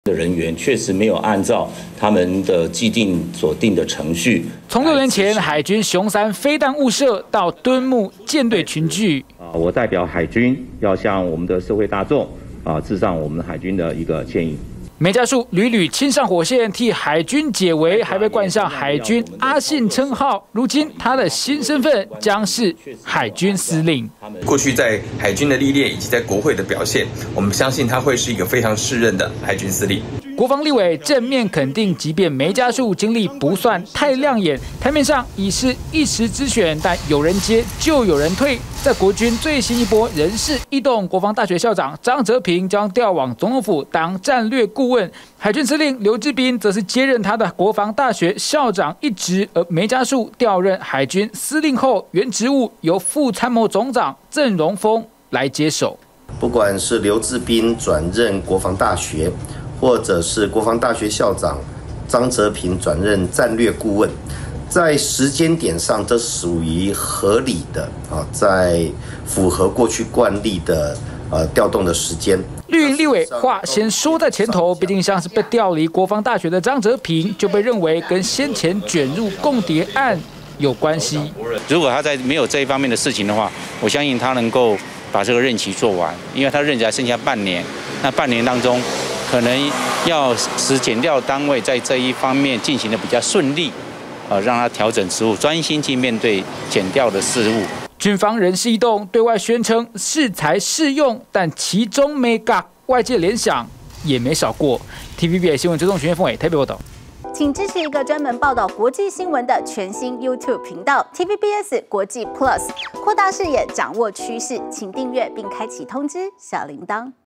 「海軍阿信」梅家樹接司令!國軍人事大地震｜TVBS新聞
國軍高階將領人事，今天有重要異動！國防大學校長張哲平，將調任總統府戰略顧問，遺缺由海軍司令劉志彬接任；至於海軍司令，則是昔日「最年輕中將」，也是號稱海軍危機處理專家的梅家樹。